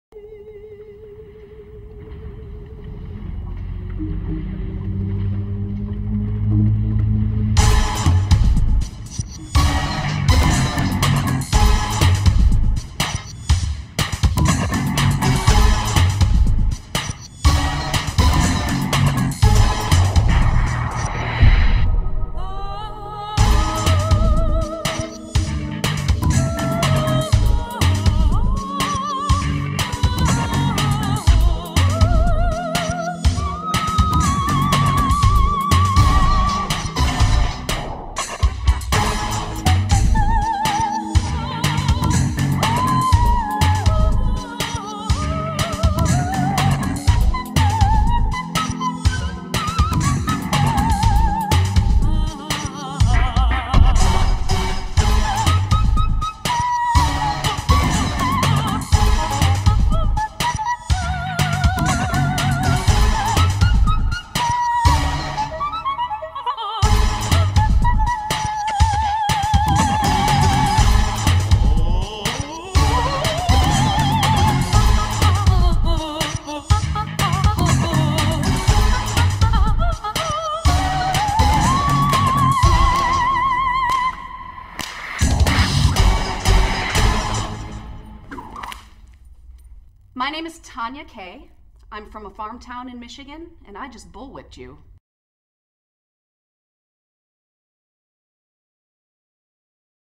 Tonya Kay
Clips from a variety of Tonya Kay performances that display the breadth of her performance capacity. Skilled in dance, fire arts, and whip handling and more.

[Re-edited after Salient Media declared that a performer could not use their video to promote themselves. ]